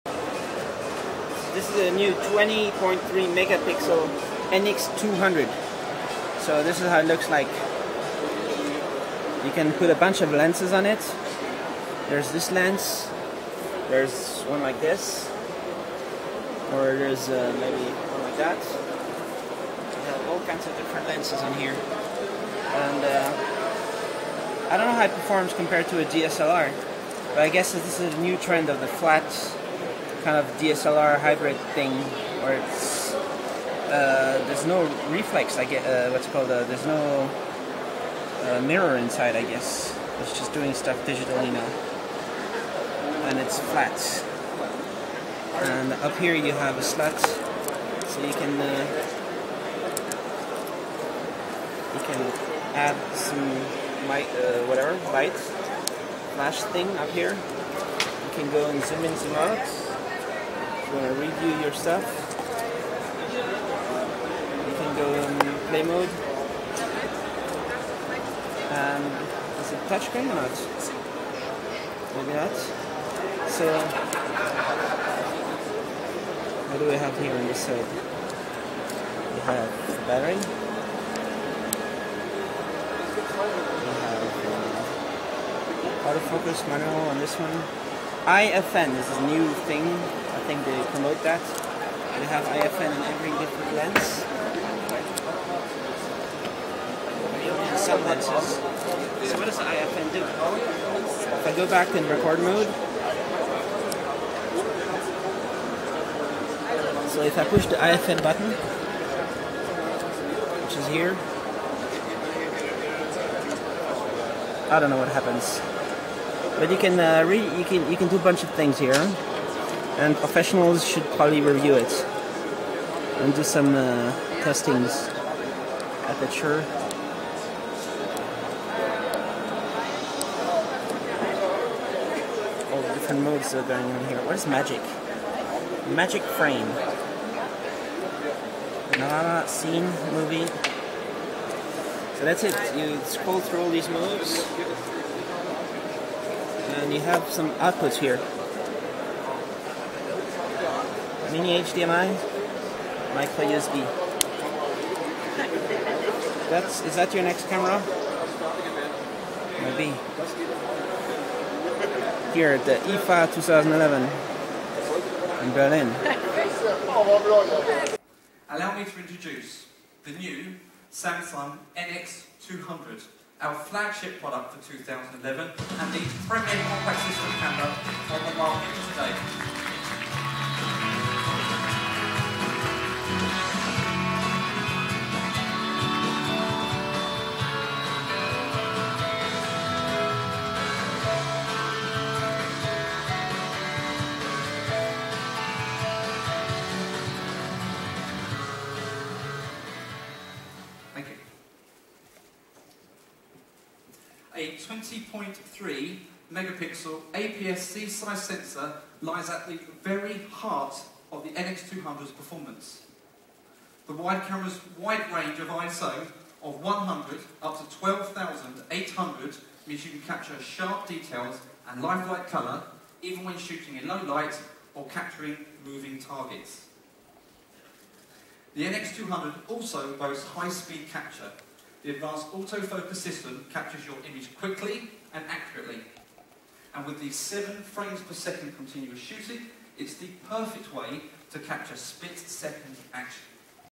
Samsung NX200 camera, 20.3Megapixels APS-C sensor, interchangeable lenses
Samsung releases their new slim mirror-less DSLR-alternative of a photo camera with interchangeable lenses and a bunch of new camera UI features.